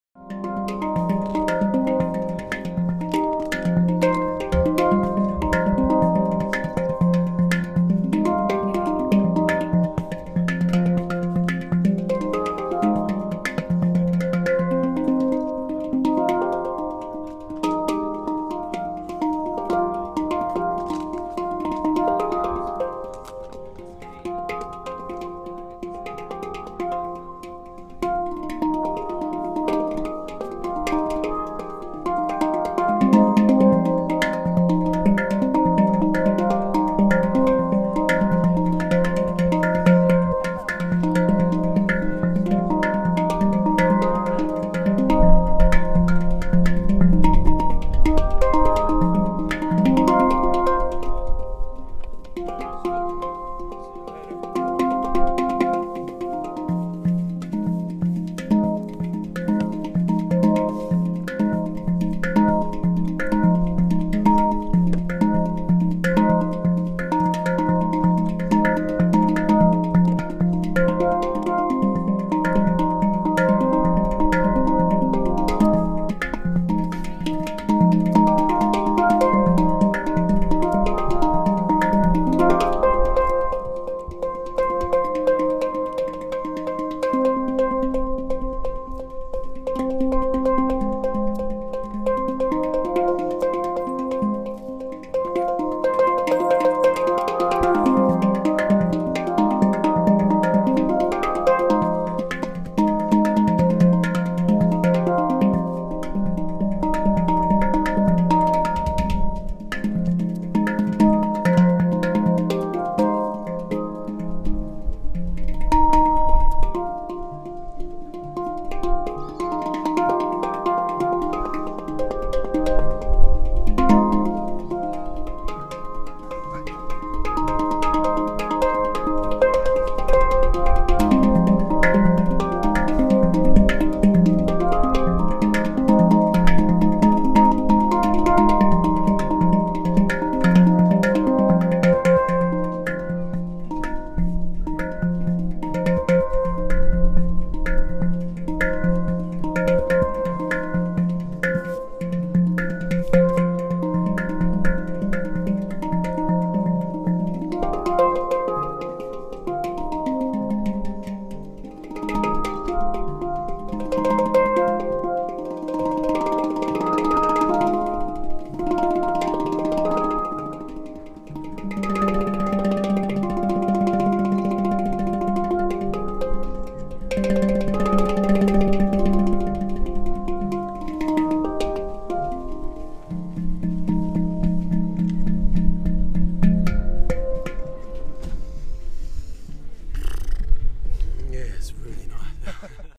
Meraki F Pygmy
Watch in 720p HD for best sound quality. First time trying a Meraki in F Pygmy scale at HangoutUK 2015. Fantastic instrument!